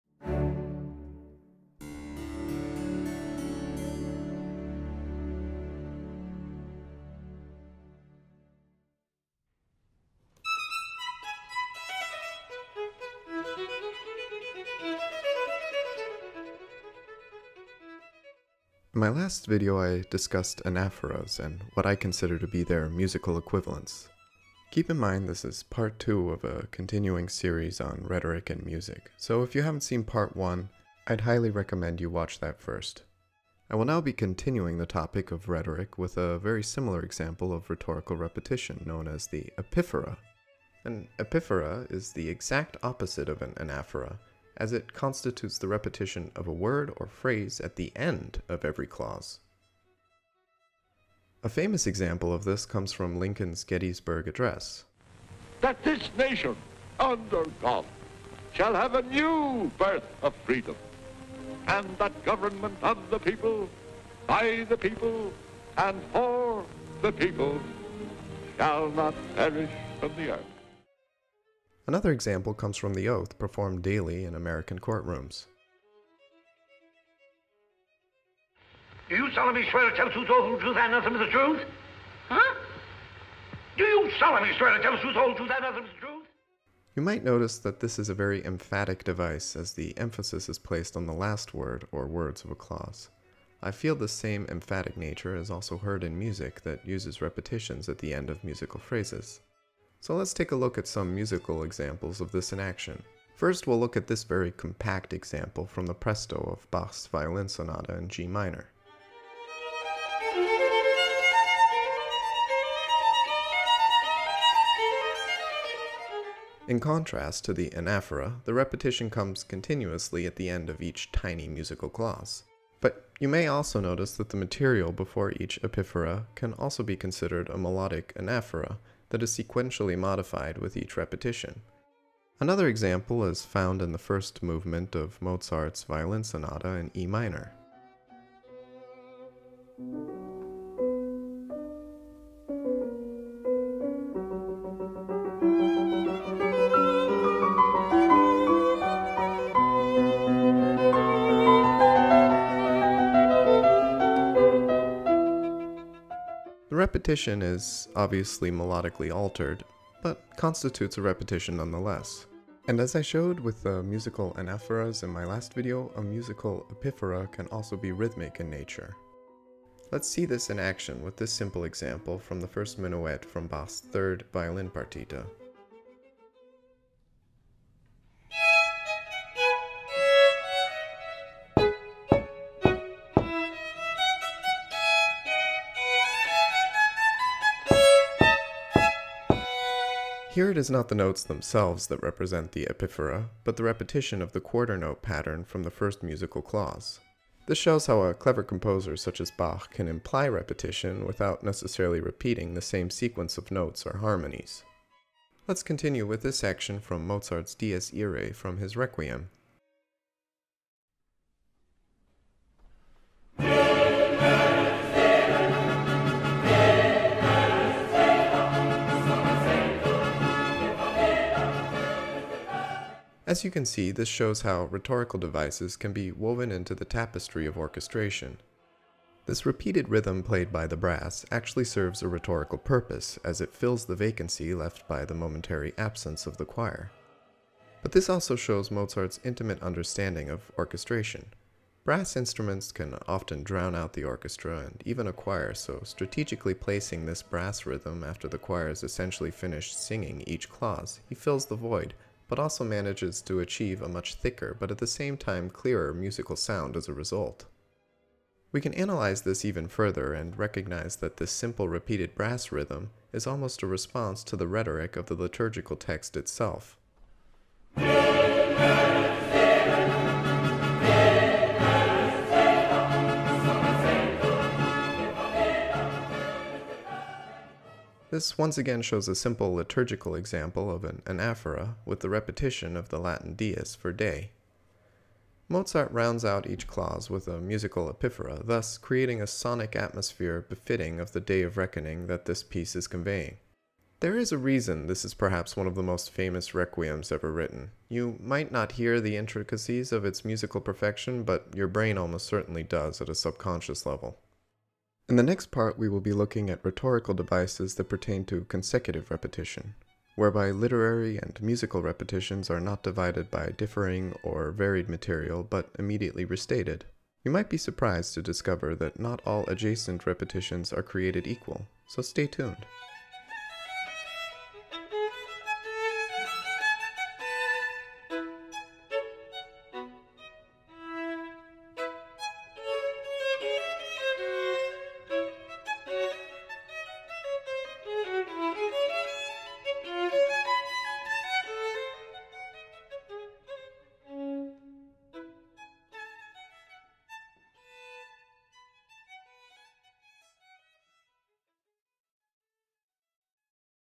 Principles of Music: Rhetoric Part II
In this video I continue to discuss how rhetorical devices can be applied to music.  I initially considered putting what I discuss here into Part I, but I didn't want to overload the viewer with too many rhetorical devices at once.

A special thanks as always to musopen.org and imslp.org for offering free public domain sheet music and recordings online.